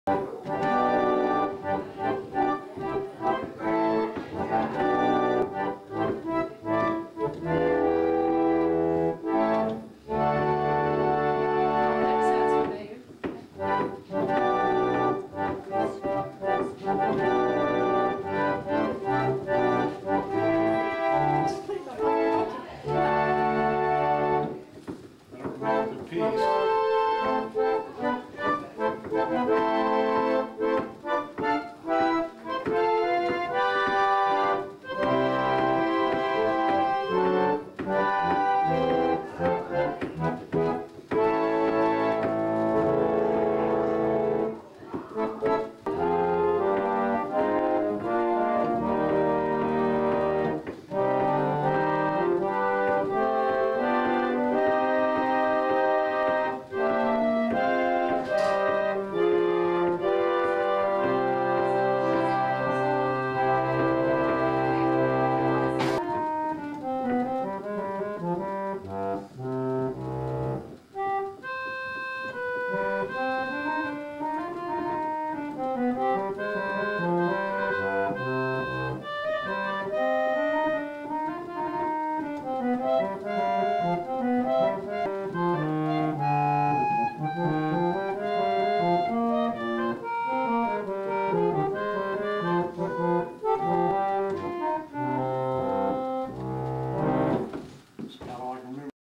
Knoxville Organist on 1909 Shipman Reed Organ
various pieces to demonstrate the Shipman Organ